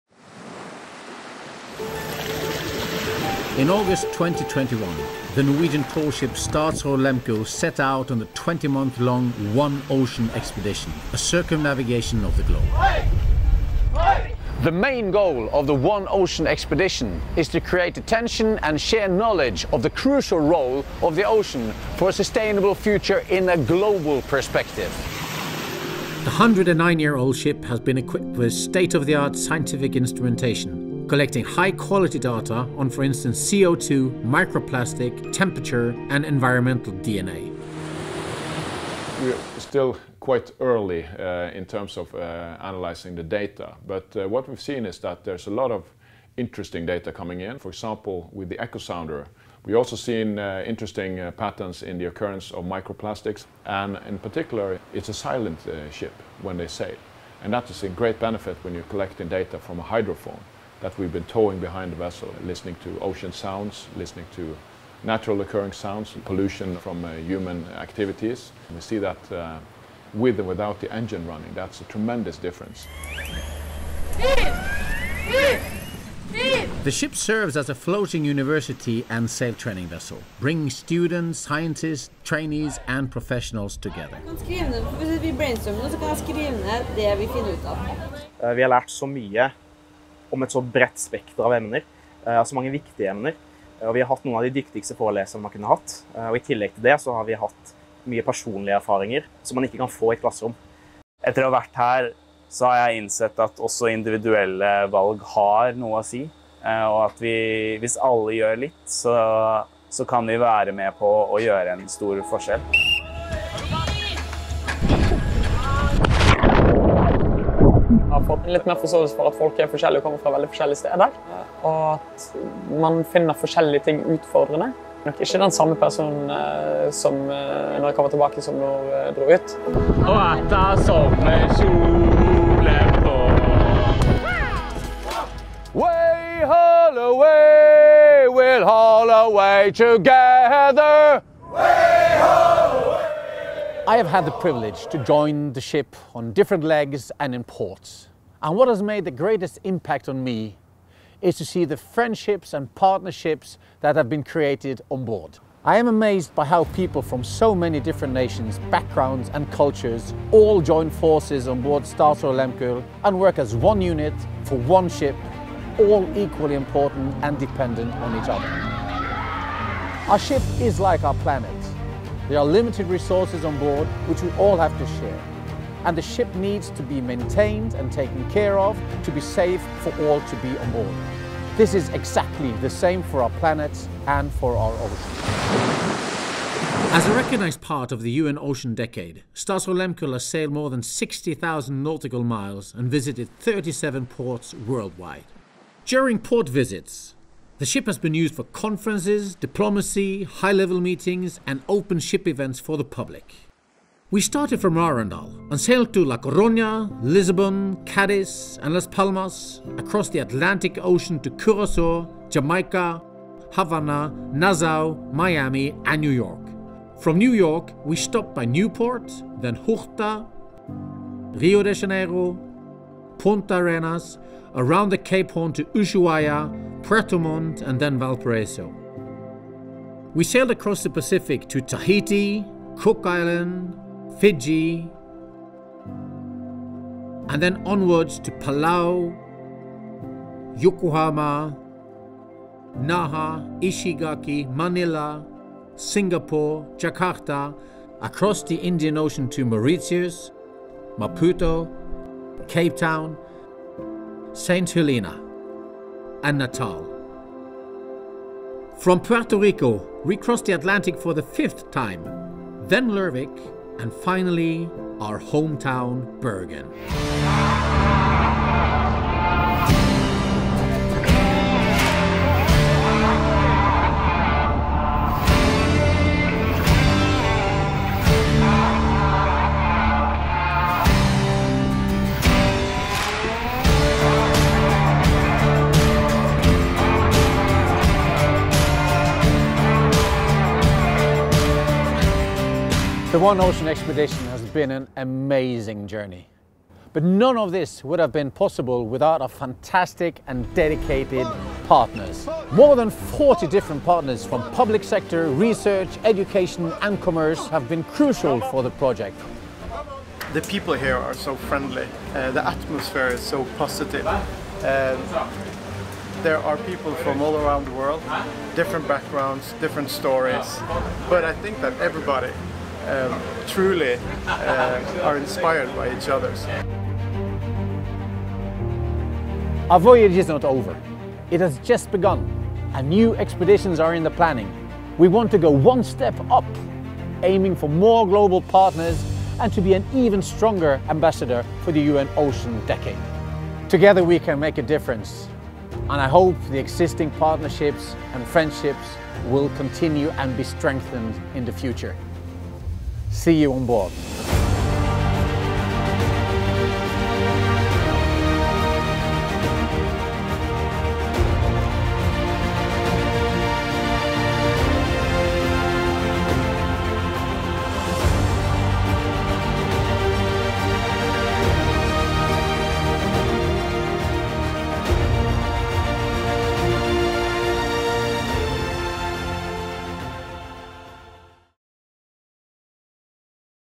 Statsraad Lehmkuhls One Ocean Expedition
Jordomseilingen One Ocean Expedition er avsluttet, og Statsraad Lehmkuhl er trygt tilbake i Norge etter en fantastisk hjemkomst og folkefest i Bergen 15. april 2023.
Etter å ha seilt 60 000 nautiske mil og besøkt 37 ulike havner over hele kloden kan vi med trygghet si at vi har lykkes med målet for ekspedisjonen: Å skape oppmerksomhet og dele kunnskap om havets avgjørende rolle for en bærekraftig framtid i et globalt perspektiv.
En stor takk til våre over 40 partnere fra offentlig sektor, forskning, utdanning og næringsliv som har gjort eventyret mulig! Vi håper at vennskapene og partnerskapene vil fortsette når vi nå planlegger for en ny ekspedisjon i 2025. Eventyret er ikke over!
The circumnavigation One Ocean Expedition is over, and Statsraad Lehmkuhl is back in Norway after a wonderful homecoming and folk festival in Bergen on 15 April 2023.
After sailing 60,000 nautical miles and visiting 37 different ports around the globe, we can safely say that we have succeeded in the goal of the expedition: To create attention and share knowledge about the ocean's crucial role for a sustainable future in a global perspective.
A big thank you to our over 40 partners from the public sector, research, education and commerce who have made the adventure possible! We hope that the friendships and partnerships will continue when we now plan for a new expedition in 2025. The adventure is not over!